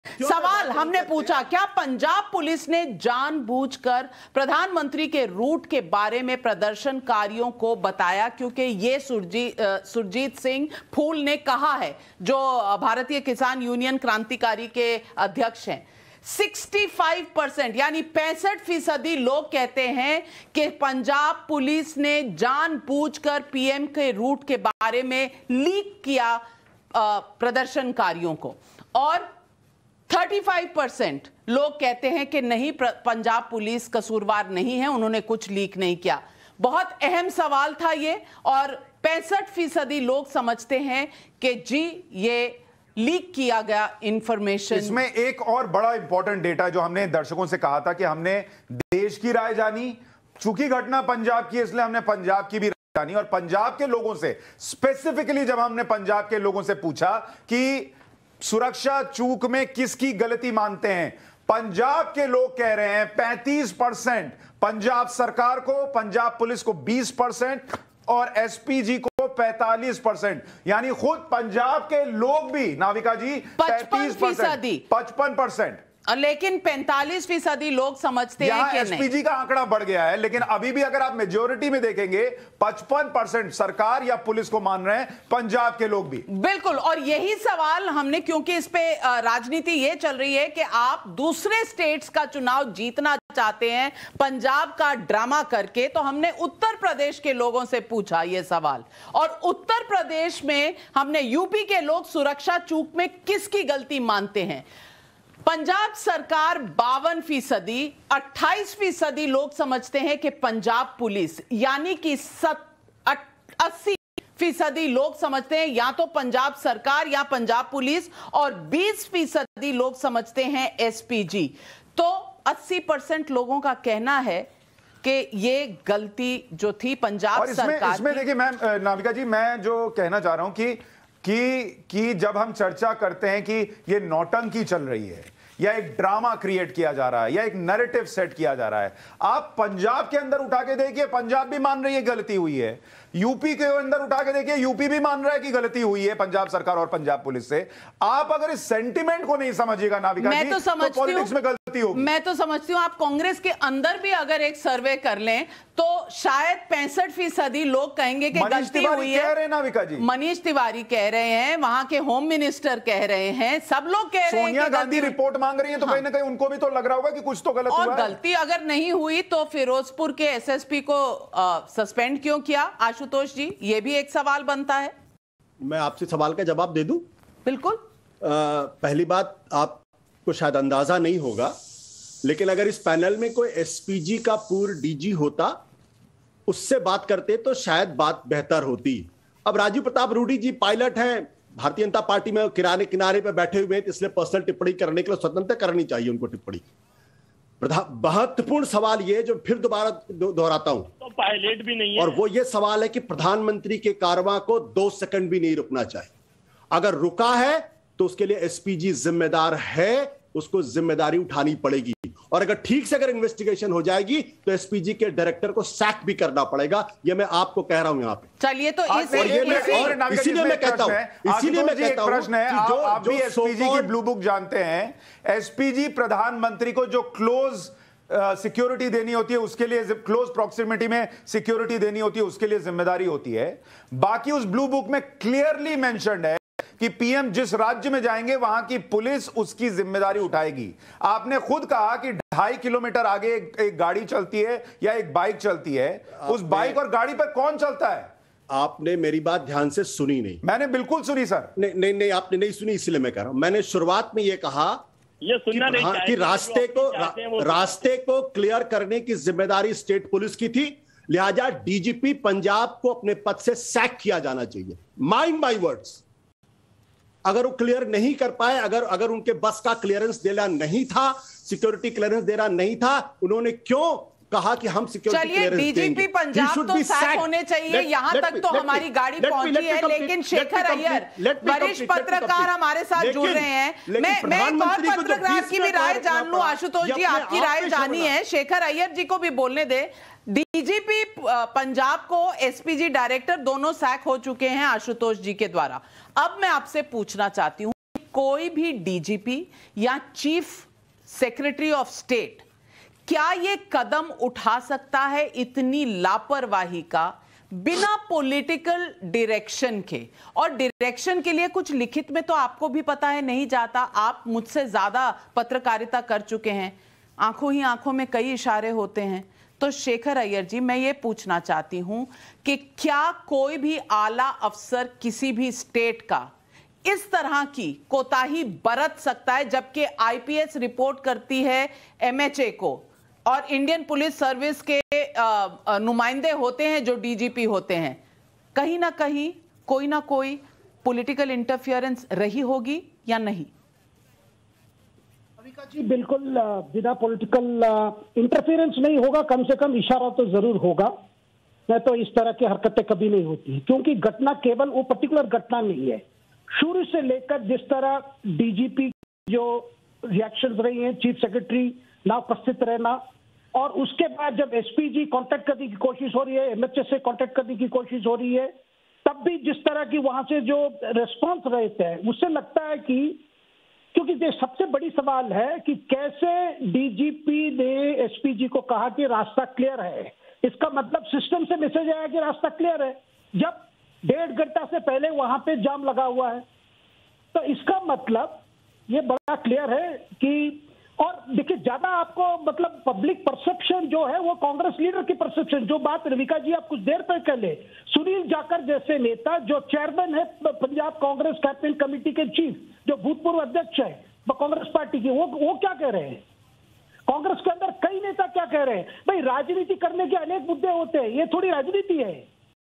TNN Survey: क्या Punjab Police ने प्रदर्शनकारियों को जानबूझकर बताया PM का रूट? PM Security Breach
PM Security Breach | TNN Survey: क्या पंजाब पुलिस ने प्रदर्शनकारियों को जानबूझकर बताया PM का रूट? जानिए जनता जनार्दन का जवाब!